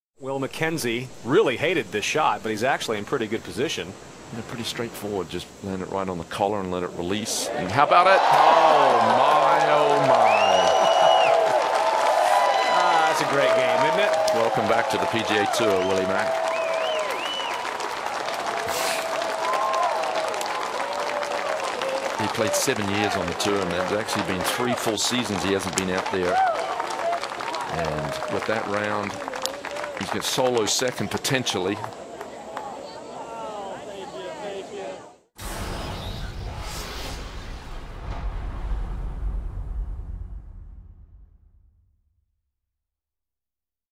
MacKenzie eagles No. 18 in Round 4 of the Chiquita Classic
In the final round of the 2013 Chiquita Classic, Will MacKenzie holes a 43-foot chip shot on the par-5 18th hole for an eagle three.